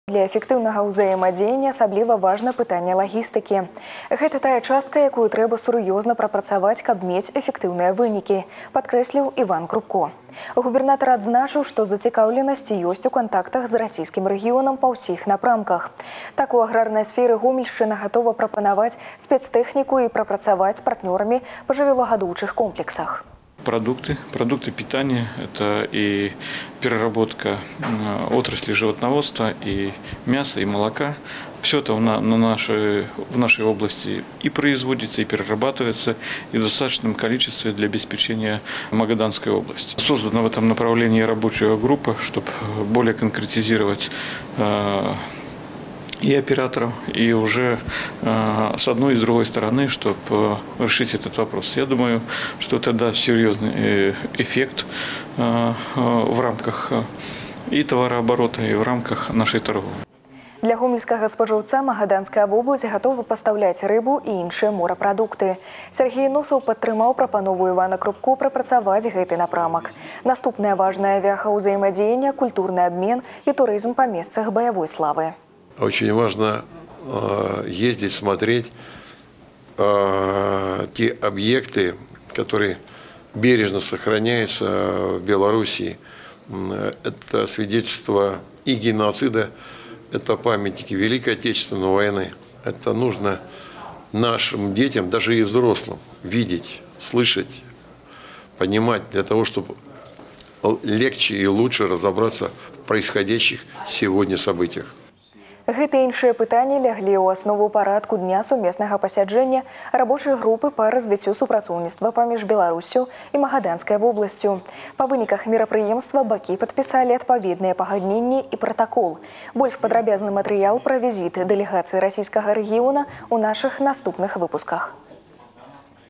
Магаданская область готова на Гомельщину поставлять рыбу и другие морепродукты
На Гомельщину прибыла делегация Магаданской области.

Гомельскую и Магаданскую области разделяют тысячи километров. Поэтому для эффективного взаимодействия особенно важен вопрос логистики. «Это та часть, которую нужно серьезно проработать, чтобы иметь эффективные результаты»,  — подчеркнул Иван Крупко. Губернатор отметил, что заинтересованность есть в контактах с российским регионом по всем направлениям. Так, в аграрной сфере Гомельщина готова предложить спецтехнику и поработать с партнерами по животноводческим комплексам.